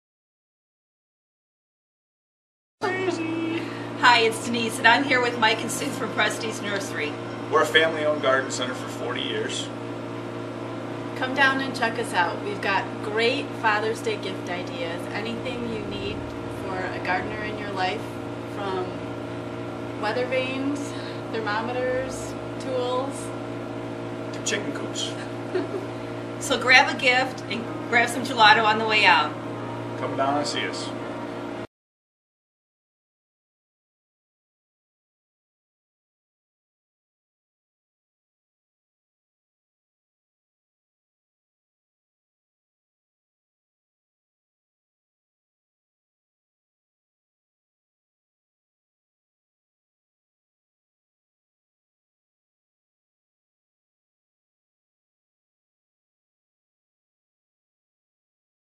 Chicago Food Trucks
Sweet Ginger's Gelato is teaming up with Prestige Nursery this summer to bring to you the finest gelato at the family run 8 acre nursery and garden center in West Chicago.

At Prestige Nursery you are going to find much more than at your run-of-the-mill big box store, along with unmatched customer service.